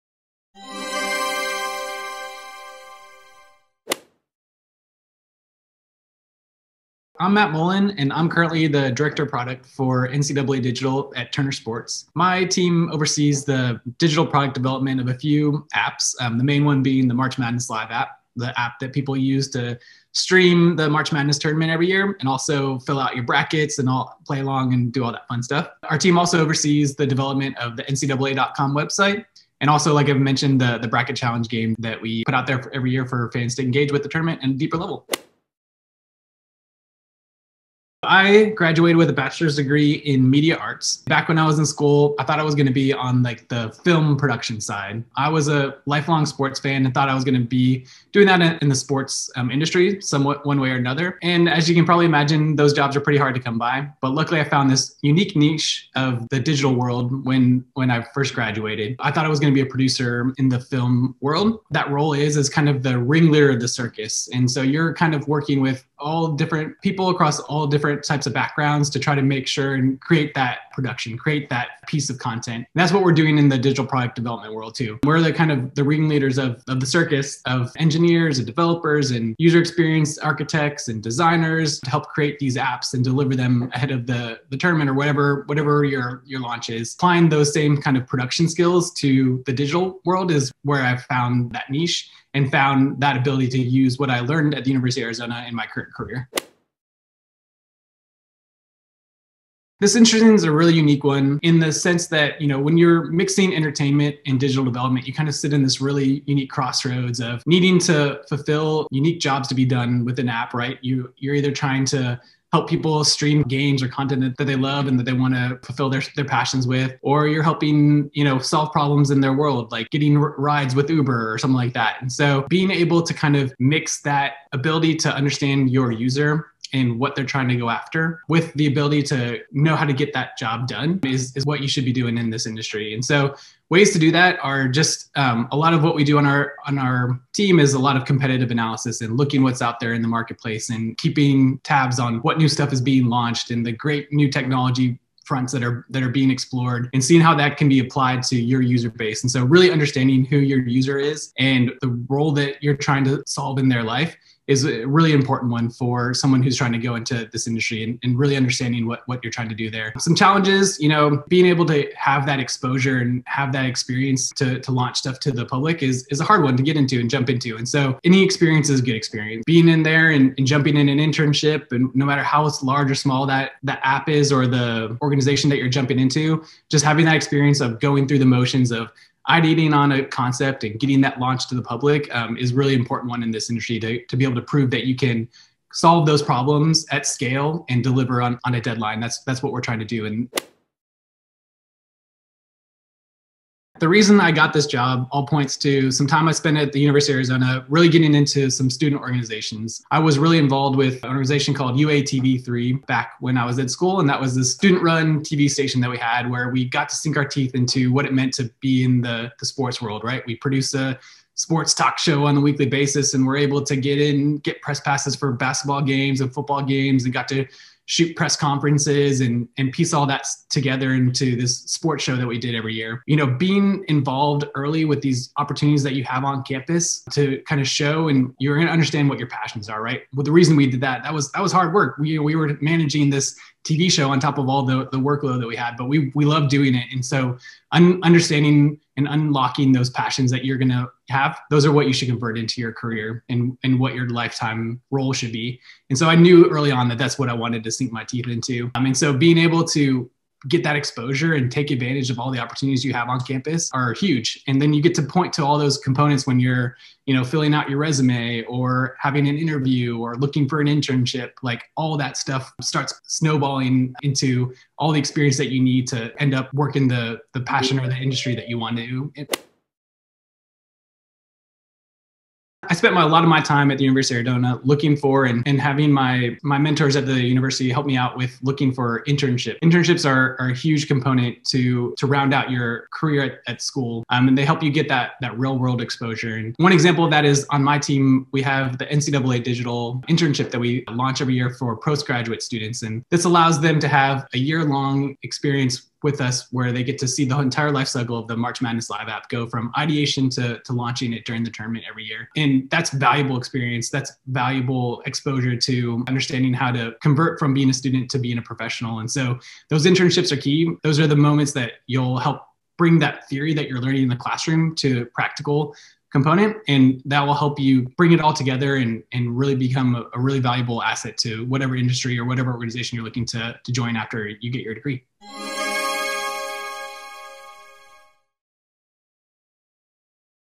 UA alum Matt Mullen shares career advice for students interested in arts and media
In this video Matt Mullen shares career advice and recommendations for students interested in the field of arts and media.

Wildcats@Work is a collaboration between the University of Arizona Alumni Association and Student Engagement & Career Development (SECD).

To learn more about programs and services by SECD, please